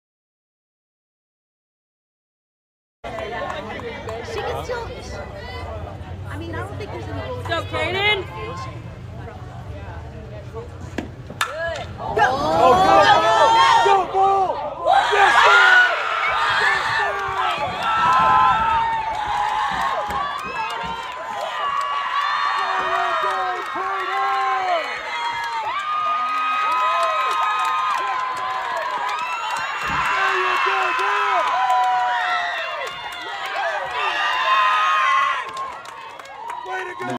Tie ball game 2 Run HR
4A State Playoffs Super Regional championship game against defending state Champs West Laurens pitcher Tannah Cobb ‘24 forced the “if necessary” game